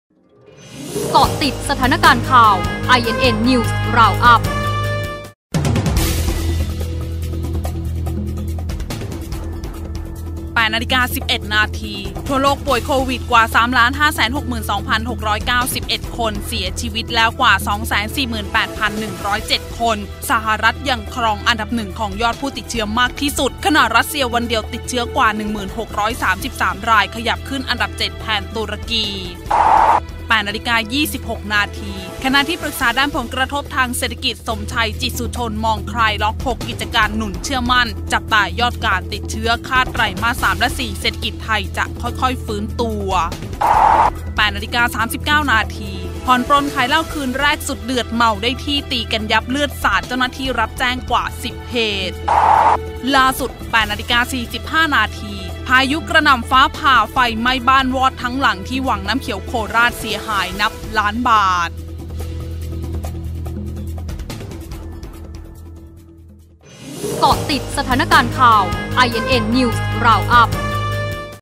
พายุกระหน่ำฟ้าผ่าไฟไหม้บ้านวอดทั้งหลัง : เกาะสถานการณ์ 09.30 น. (04/05/63)
08.11 น.ป่วยโควิดทั่วโลกกว่า3.5ล.ตายกว่า2.4แสน
08.26 น."สมชัย"มองคลายล็อกหนุนเชื่อมั่น
08.39 น.เหล้าเดือด!เมาแล้วซ่าคืนเดียววิวาทนับ10
08.45 น.พายุกระหน่ำฟ้าผ่าไฟไหม้บ้านวอดทั้งหลัง